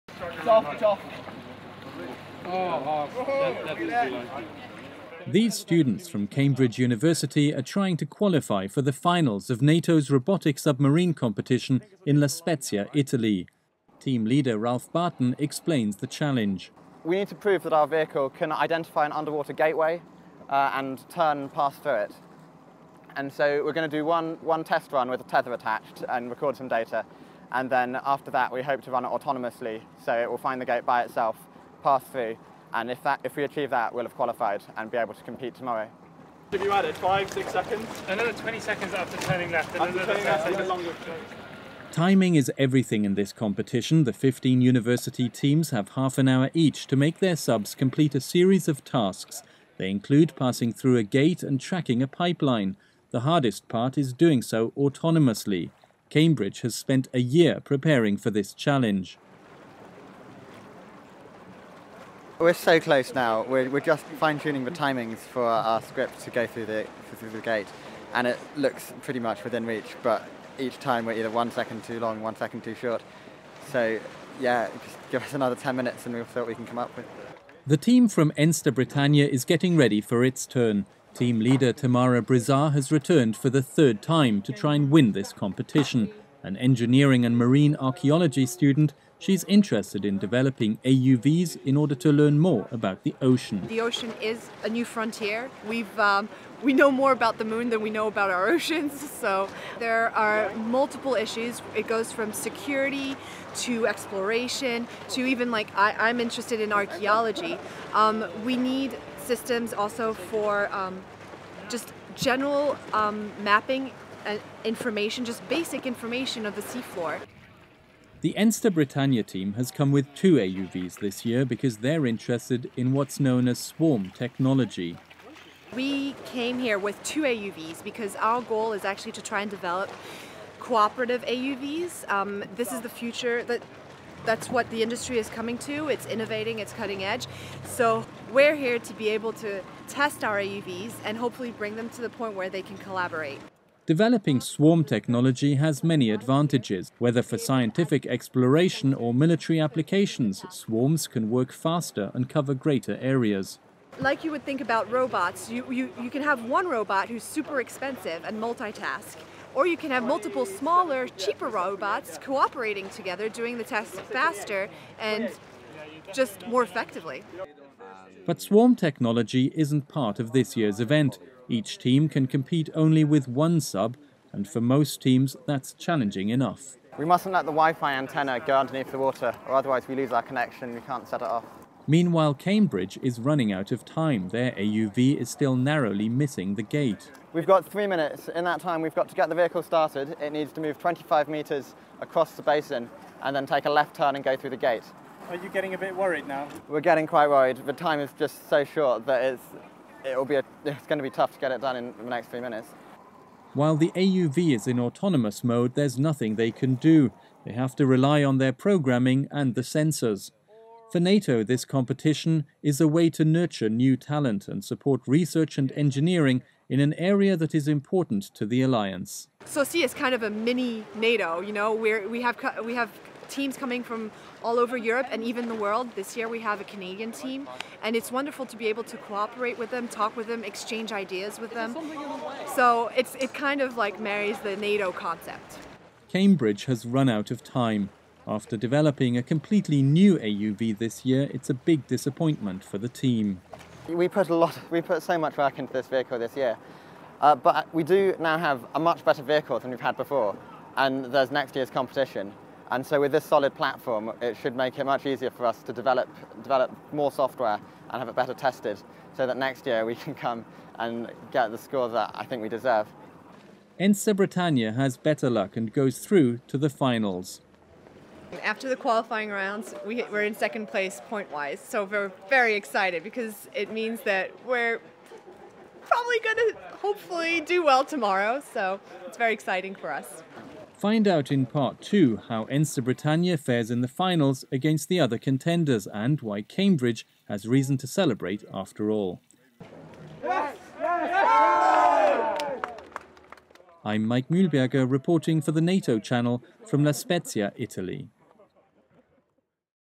NATO's Underwater Challenge - Part 1/2
At its Center for Maritime Research and Experimentation NATO supports the next generation of researchers and engineers by hosting a Robo-Sub competition. 15 University teams put their Autonomous Underwater Vehicles to the test for a place in the finals. NATO Homepage